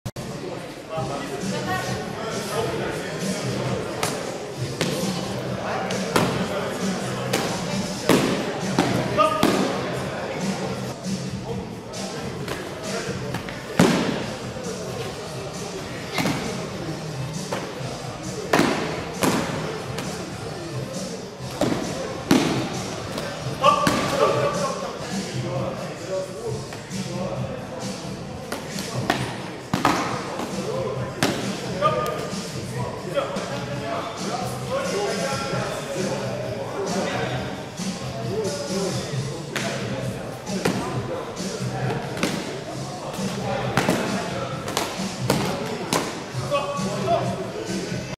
Двухручный меч 2б Полуротова
В МЦИФ Эскалибур Москва впервые проводились соревнования " КОМАНДНЫЙ КУБОК по СОВРЕМЕННОМУ МЕЧЕВОМУ БОЮ" Все номинации проводились в экипировке СМБ и со спортивными мечами и щитами